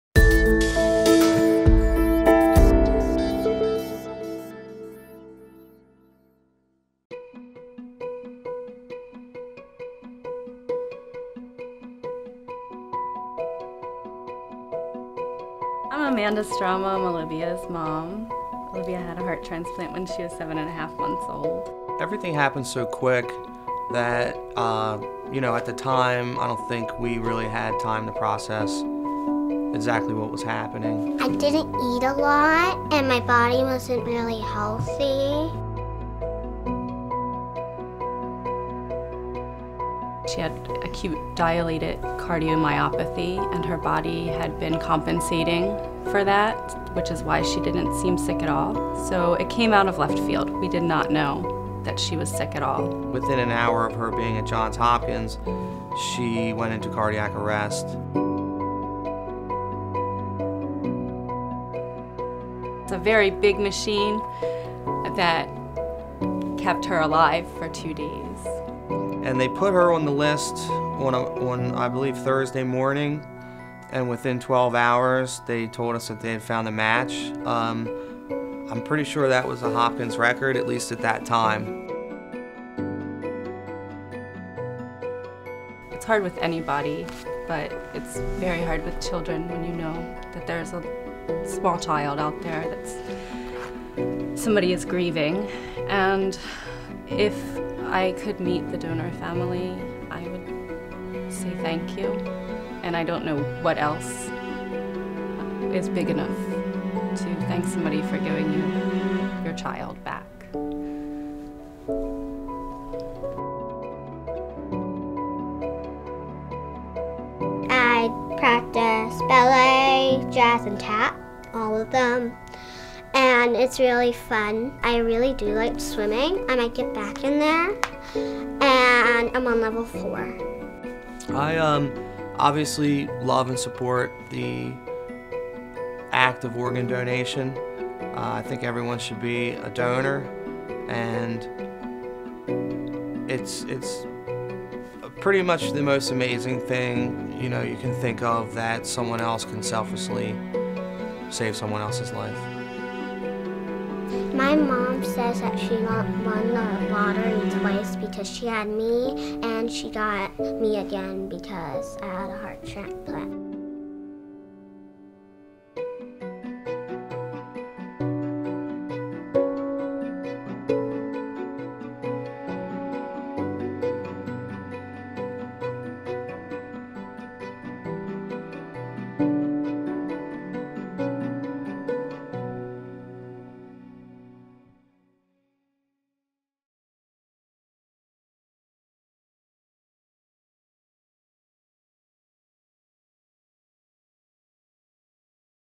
Donate Life | Olivia's Story
Olivia Strama developed acute cardiomyopathy when she was only 7 months old. She received a lifesaving heart transplant at The Johns Hopkins Hospital. Hear from Olivia and her parents, Amanda and Alex, to learn more about her story and the importance of organ donation.